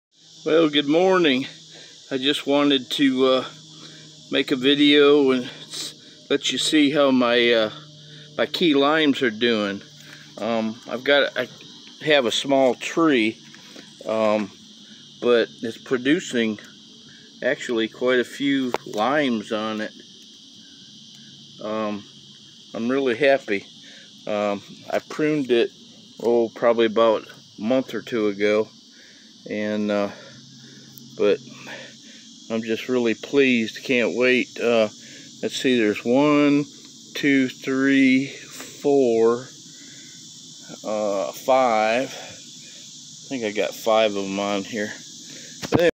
Cocktail Tree 07-15-21
Bought this cocktail tree (part Key Lime & Meyer Lemon) a couple of years ago at Lowe`s in Zephyrhills, FL and I grew my first lime last year and this year it produced five.